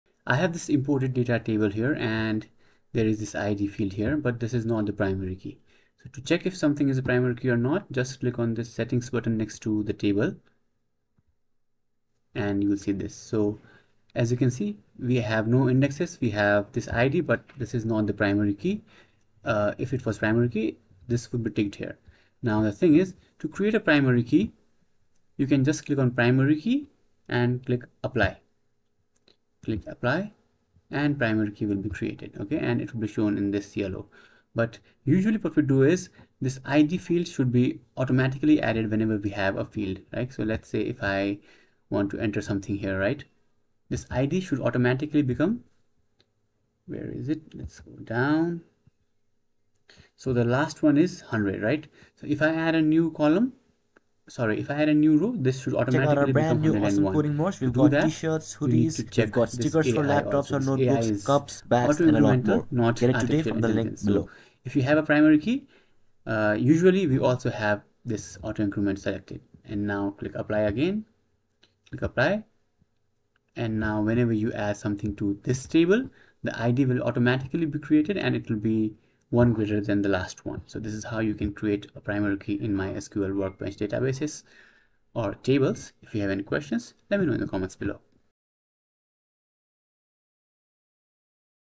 How to Add Primary Key in MySQL Workbench (2024)
In this video, I'll show you how you can add Primary Key in MySQL Workbench.
Primary key is used to uniquely identify the rows in a table. Each primary key in MySQL Workbench.
You can easily add foreign key in PhpMyAdmin without writing any SQL queries.

Steps to Add Foreign Key in PhpMyAdmin
1. Open PhpMyAdmin
2. Click on the database where you want to add foreign key
3. Click on a table in which you want to add foreign key
4. Click on Structure
5. Click on relational views
6. Select the column that you want to add as foreign key and next to it, choose referenced table and column

By the end of this video, you'll learn how you can add foreign key using phpmyadmin.